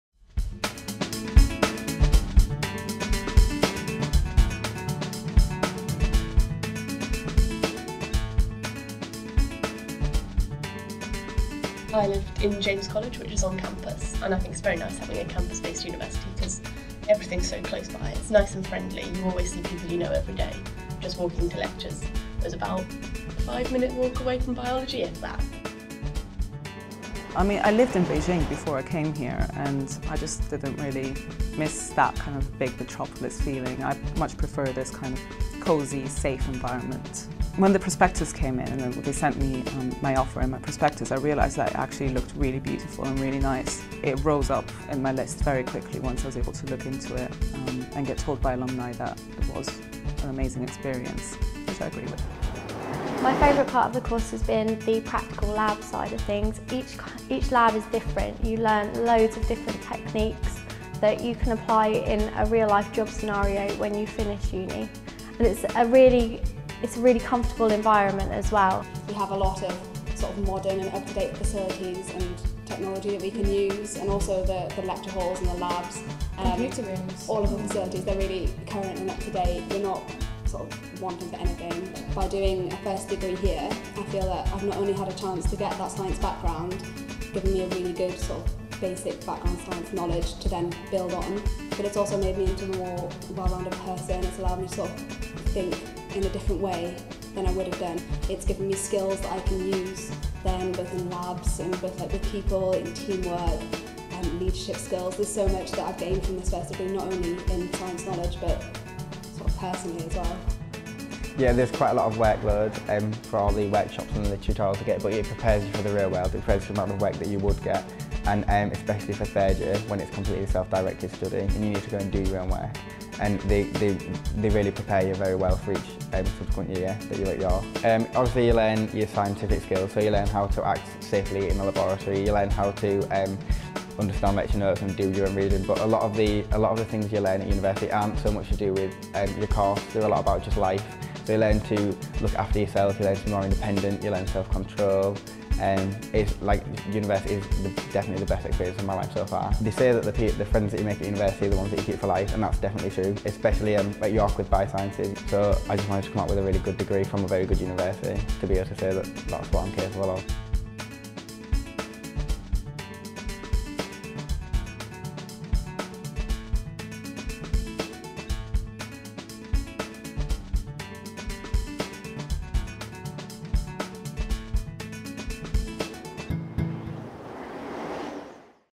Undergraduate life in the Department of Biology
Watch our short video to see what life is like as an undergraduate student in the Department of Biology at the University of York